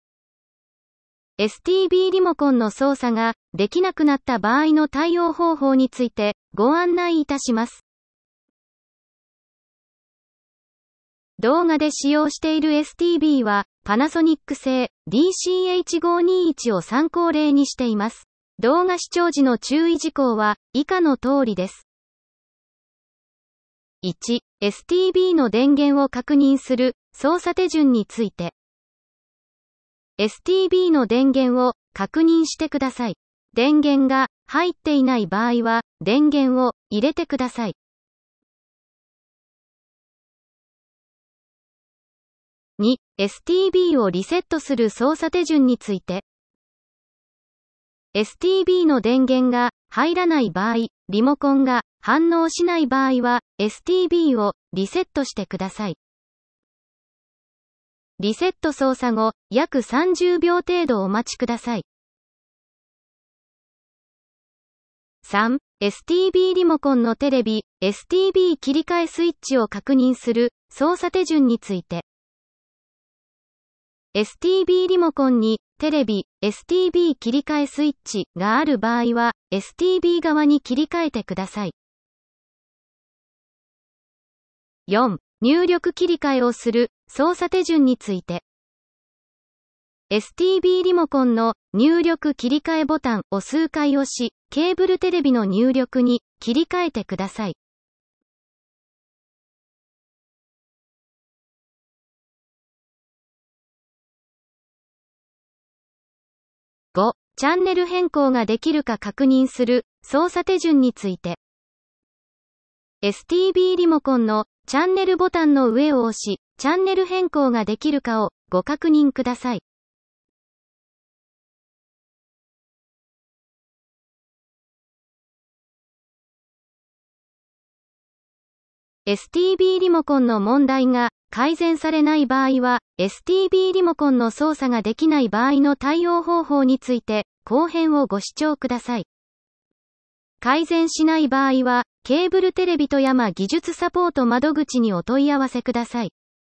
STBリモコンの操作ができない場合の対応方法について【前編】
ケーブルテレビ富山 技術サポート窓口です。

STBリモコンの操作ができない場合の対応方法について
【前編】をご案内いたします。
（こちらの動画で操作している端末は、「Panasonic社製
　TZ-DCH521」を参考例にしています）

ケーブルテレビ富山 技術サポート窓口
電話受付時間：9:00～17:00(先時間以外・土曜・日曜・祝日・年末年始は時間外受付に転送)